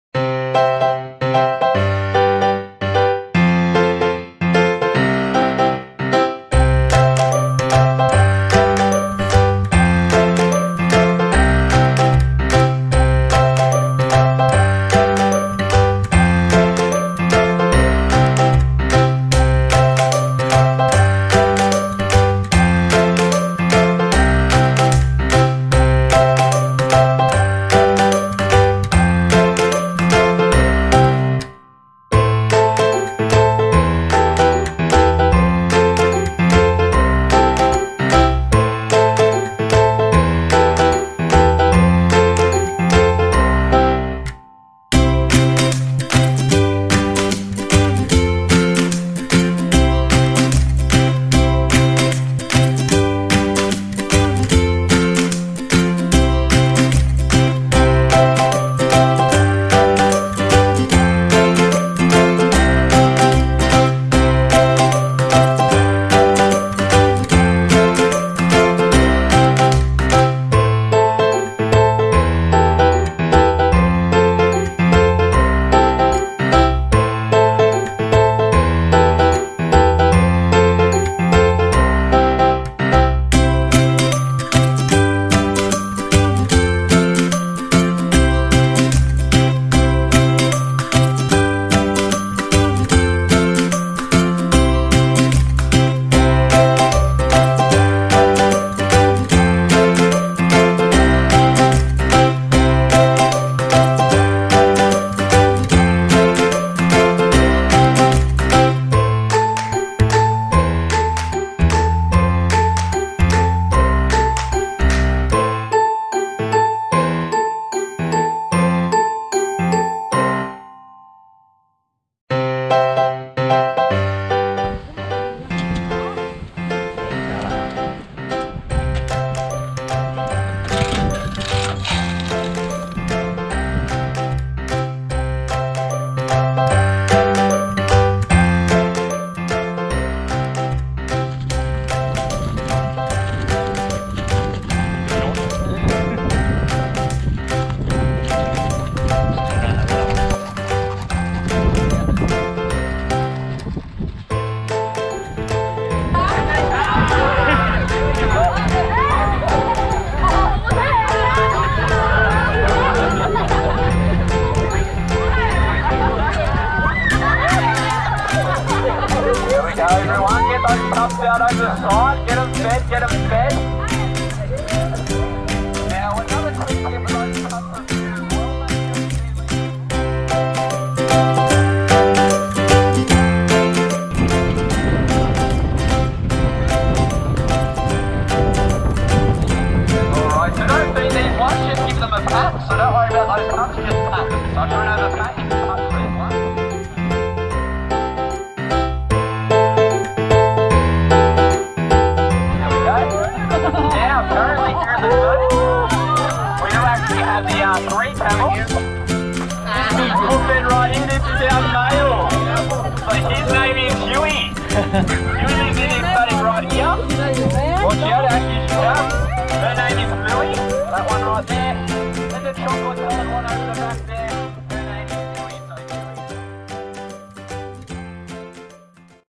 TASMANIAN ZOO (Part 2)
Du lịch trải nghiệm tại Tasmania
Tasmania được biết đến với hệ sinh thái độc nhất thế giới với các loài động thực vật duy nhất chỉ có thể tìm thấy tại hòn đảo này như Tasmanian Devil.
Mời mọi người cùng Tassie Buddy tham quan những khu vườn thú ở Tasmania nhé